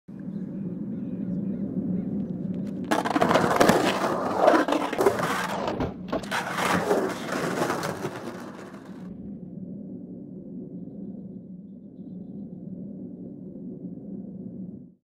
Kevin Kowalski Pivot Fakie
Kevin Kowalski charging a monster pivot fakie.
IndependentTrucks.com